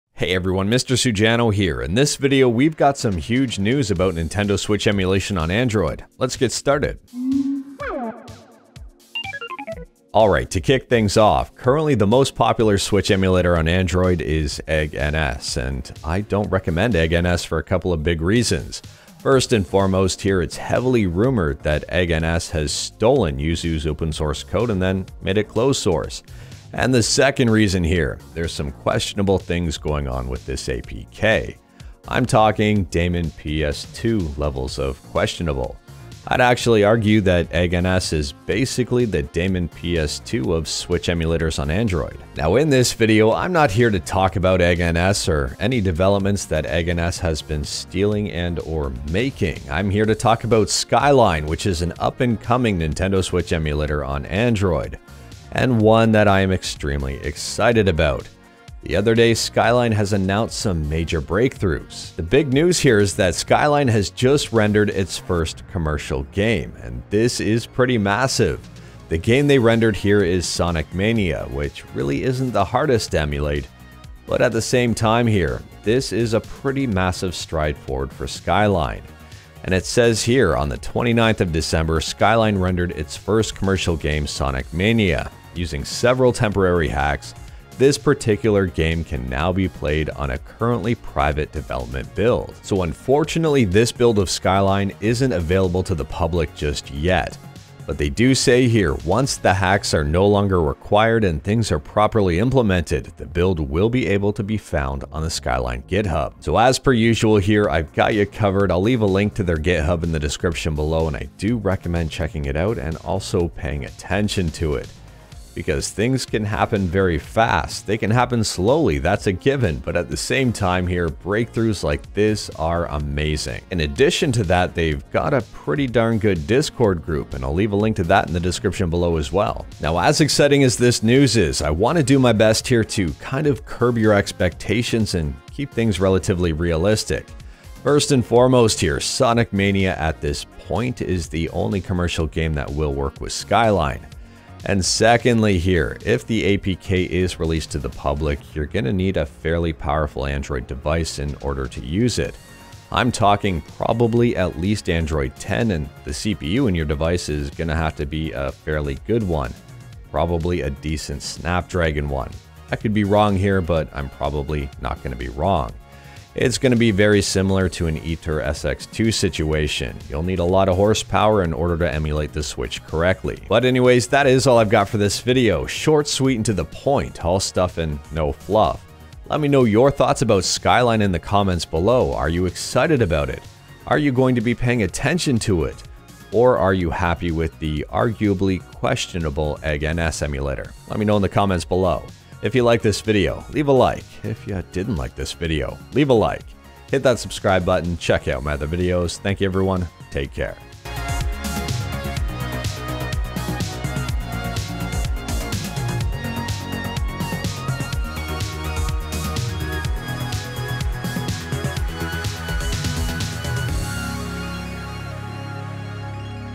Nintendo Switch Emulation on Android is heating up!
Hey all! In this video we talk about the current state of Nintendo Switch Emulation on Android - including Egg NS as well as the new breakthrough from Skyline!